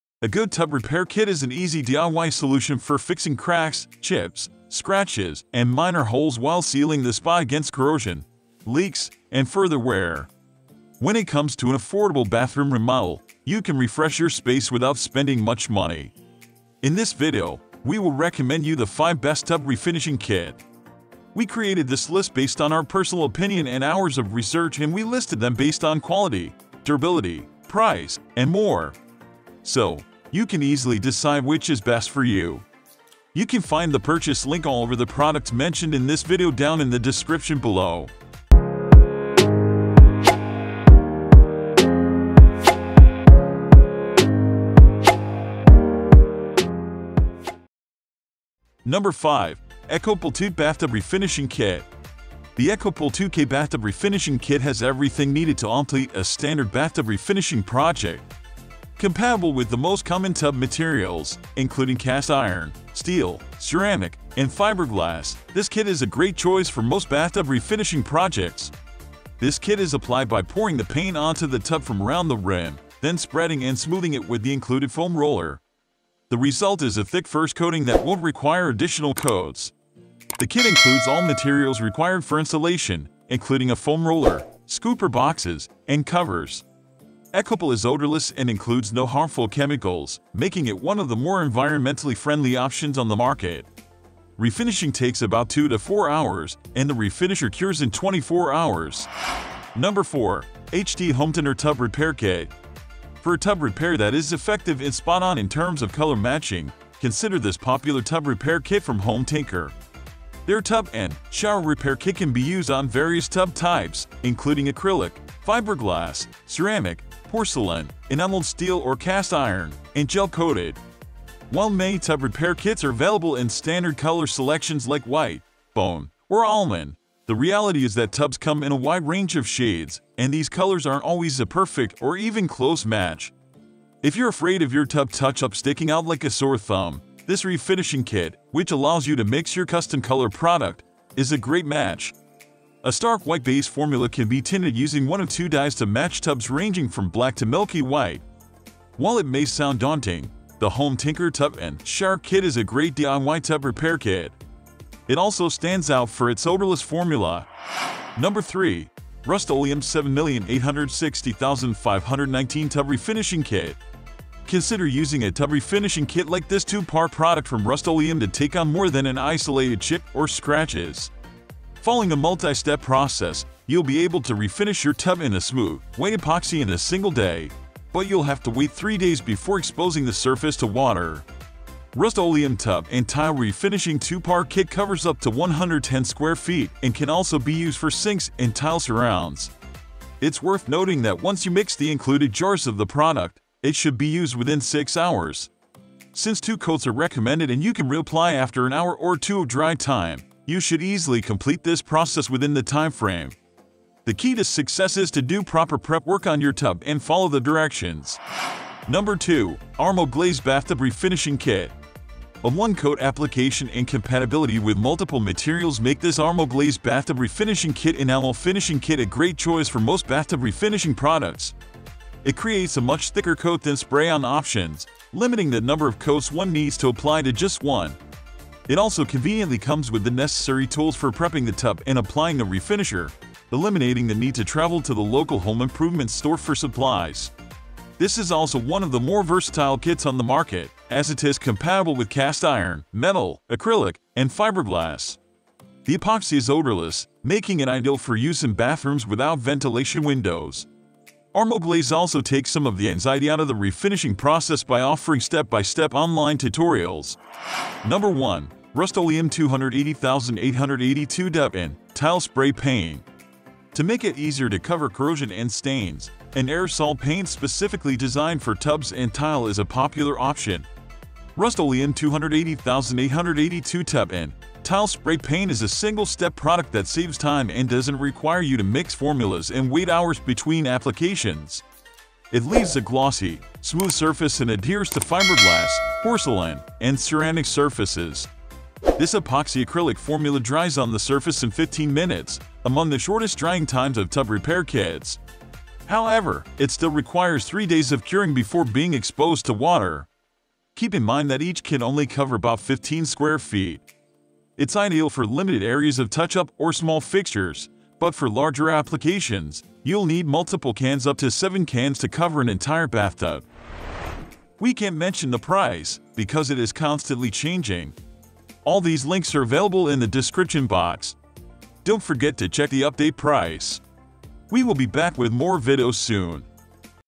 Top 5 Best Tub Refinishing Kits You can Buy Right Now [2023]
Are you Searching for the Best Tub Refinishing Kits in 2023? So now you are in the right place for getting the valuable info on Tub Refinishing Kits.

Links to the Best Tub Refinishing Kits are listed below.

0:49 Ekopel 2k Bathtub Refinishing Kit

01:39 HT Hometiner Tub Repair Kit

02:38 Rust-Oleum 7860519 Tub Refinishing Kit

03:29 ArmoGlaze Bathtub Refinishing Kit

04:18 Rust-Oleum 280882 Tub & Tile Spray Paint